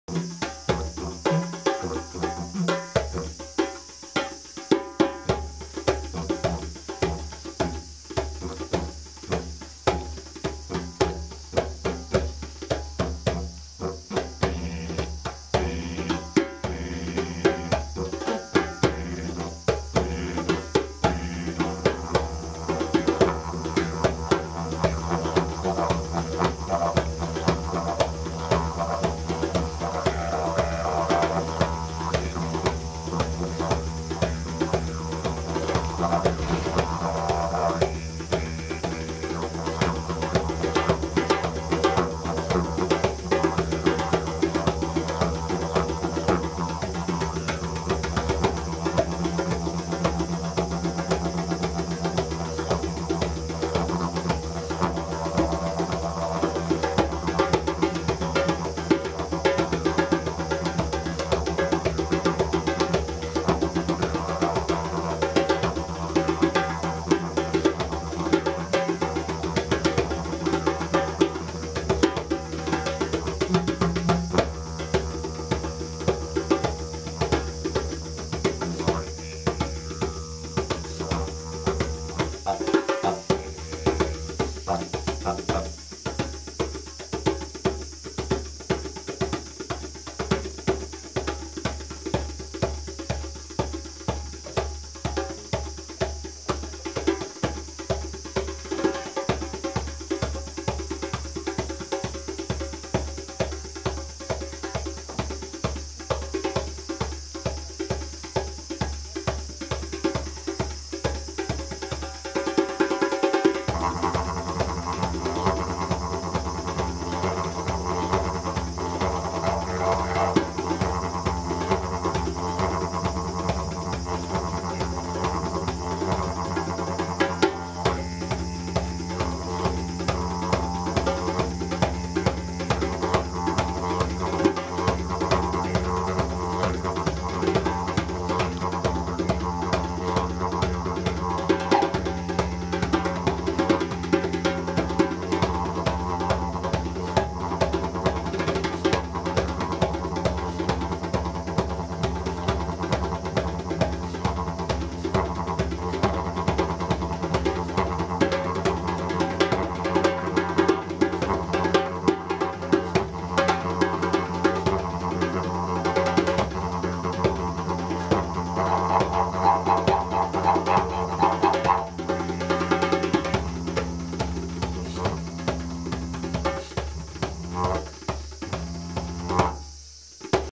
夜の雨
真夏の夜、雨の振る中でのフリーセッション。
湿度が高く、じっとしていても汗ばむほどのコンデションの中、
野外フリーセッションが実現しました。